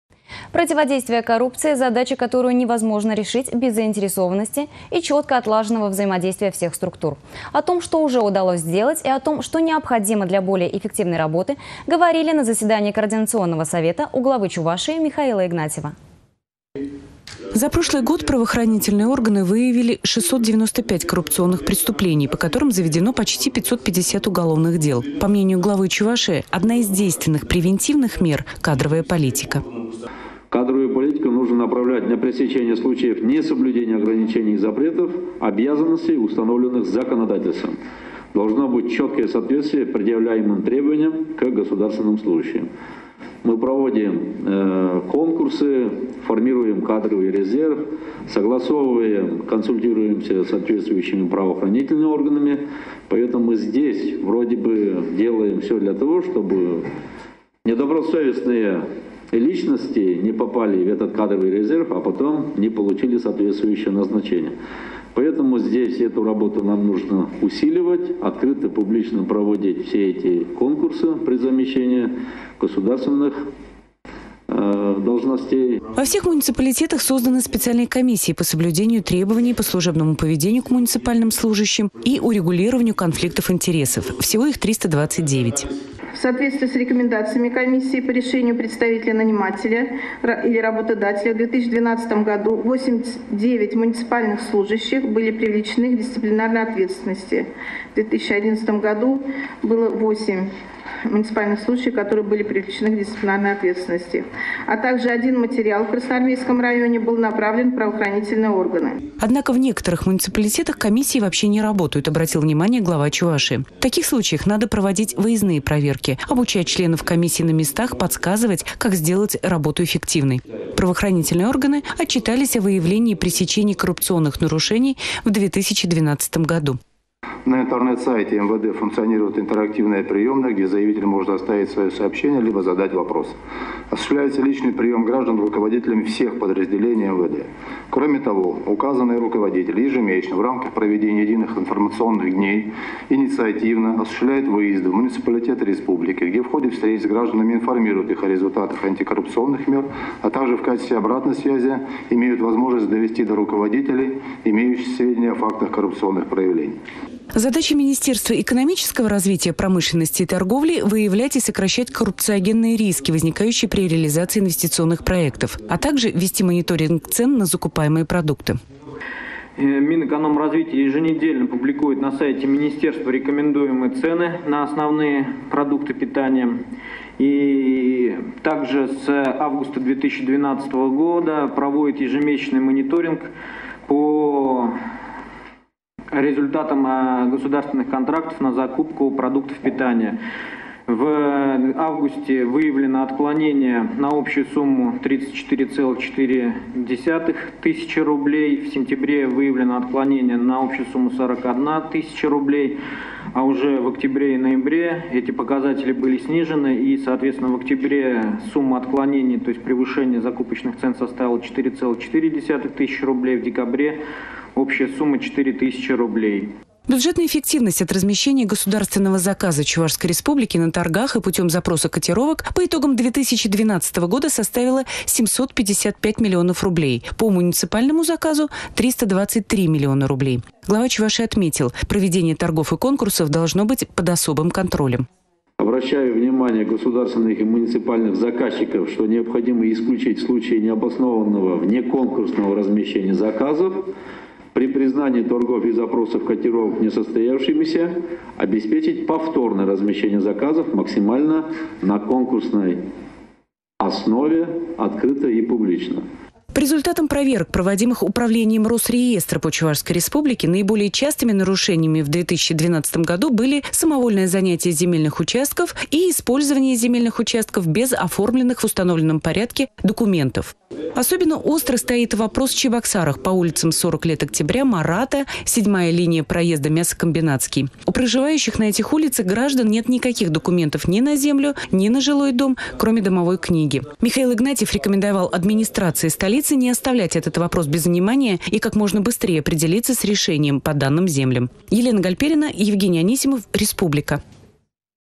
Координационный совет по противодействию коррупции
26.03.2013
Противодействие коррупции — задача, которую невозможно решить без заинтересованности и четко отлаженного взаимодействия всех структур. О том, что уже удалось сделать, и о том, что необходимо для более эффективной работы, говорили на заседании Координационного совета у Главы Чувашии Михаила Игнатьева.